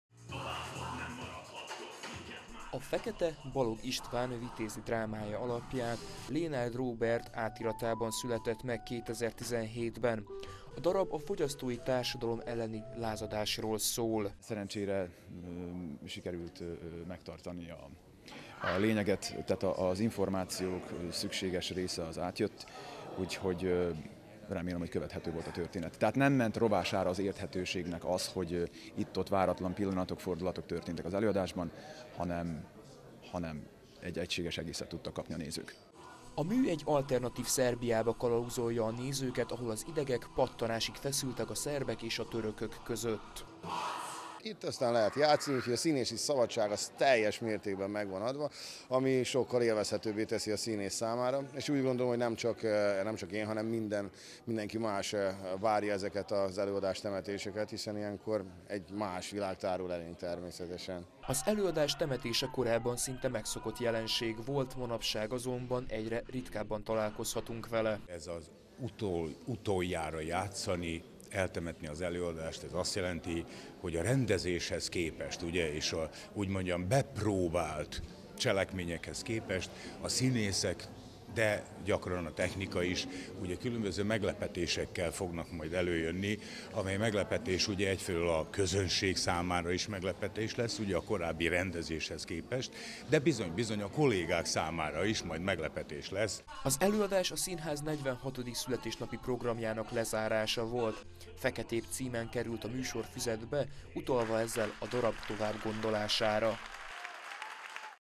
2020.01.28. - Eltemették a Fekete című előadást az Újvidéki Színház színészei
A darab ezzel lekerült a színház repertoárjáról. Nemcsak a közönséget, de a színészeket is meglepetések érték játék közben, amelyek nem szerepeltek az eredeti darabban.